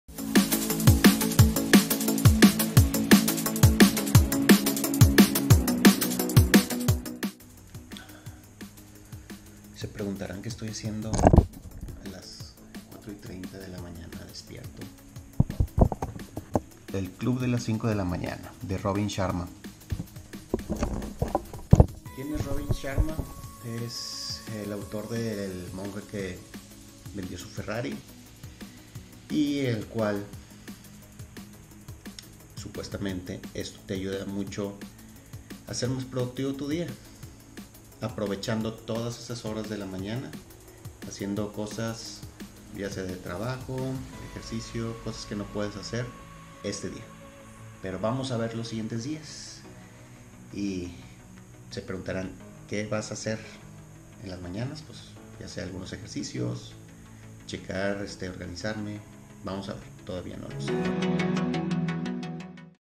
El club de las 5 de la mañana
Este video es el comienzo de poder levantarme todos los días, espero, para así poder sacar las provecho a los días y ser más productivo.